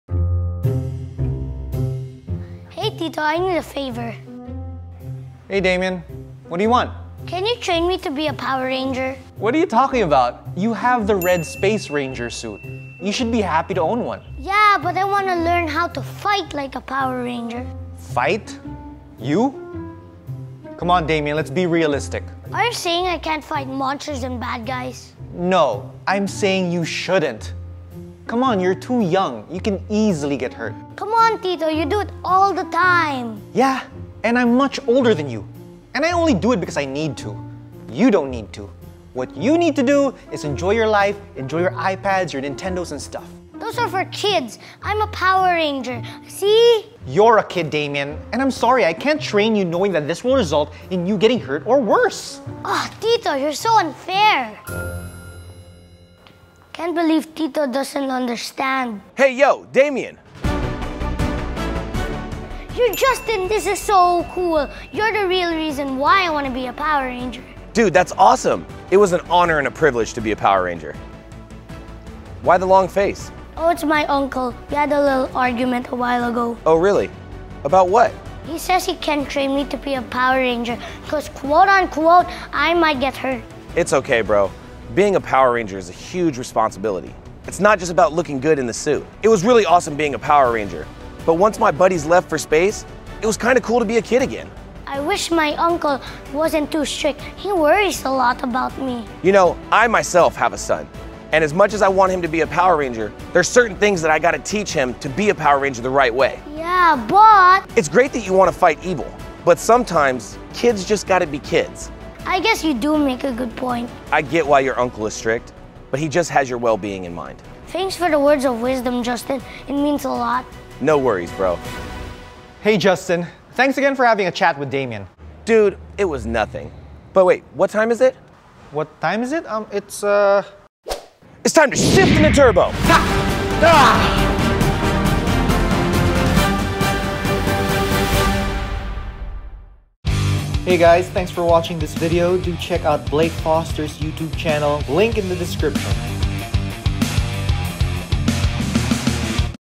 The Turbo Ranger - feat. Blake Foster [FAN FILM] Power Rangers | Super Sentai
Here's my newest fan film featuring the BLUE TURBO RANGER from POWER RANGERS TURBO, Blake Foster!